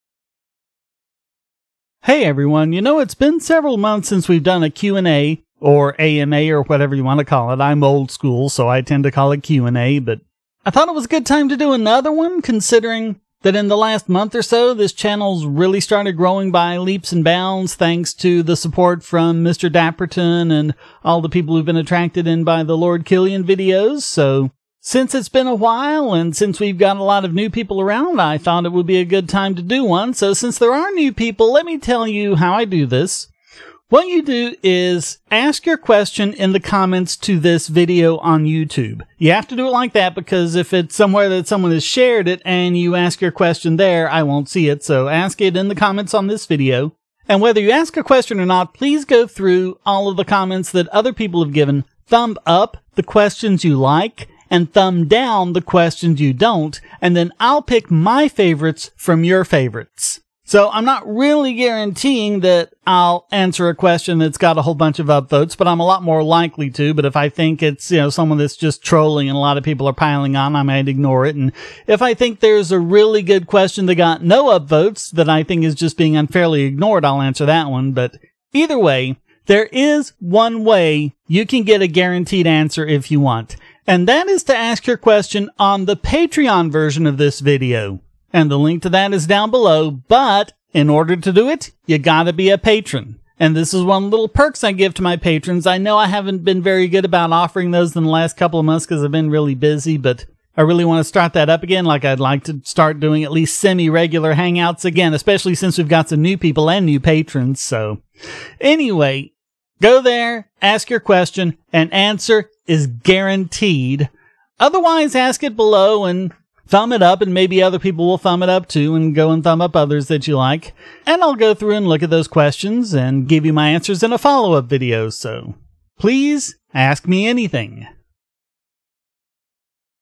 Let's do another Q&A/AMA
Let's do another Q&A, AMA, whatever you want to call it! Ask me anything in the comments, thumb up the questions you like, thumb down the ones you don't. I'll pick my favorites from among your favorites, and answer them in a follow-up video.

Or, you can GUARANTEE your question will be answered by becoming a Patron